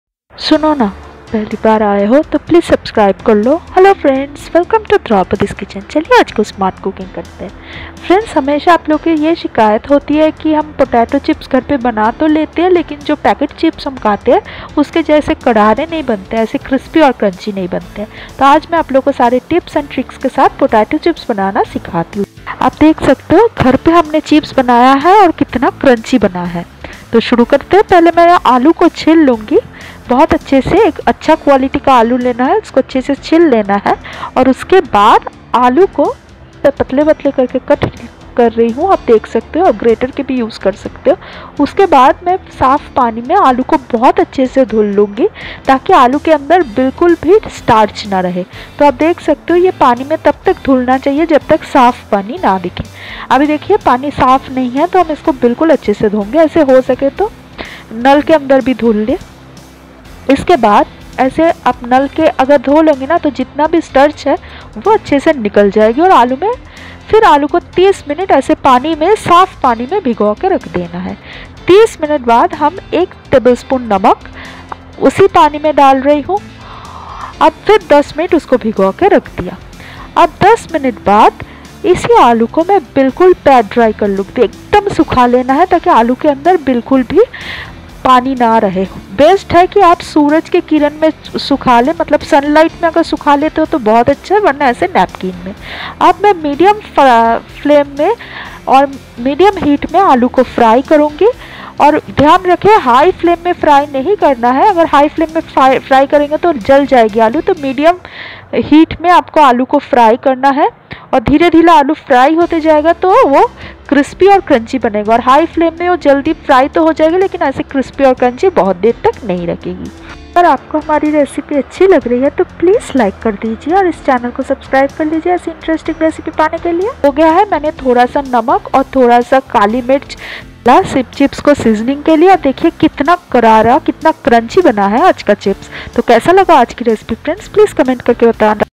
Homemade Potato chips in 10 min/Crispy Potato Chips Recipe with Tips & Tricks -Draupadi's KitchenDk
A potato chip is a thin slice of potato that has been either deep fried or baked until crunchy. They are commonly served as a snack, side dish, or appetizer

👨🏽‍🍳 Cooking Draupadi's Kitchen DK recipes? 👩🏽‍🍳
📸SHOW US YOUR PHOTOS!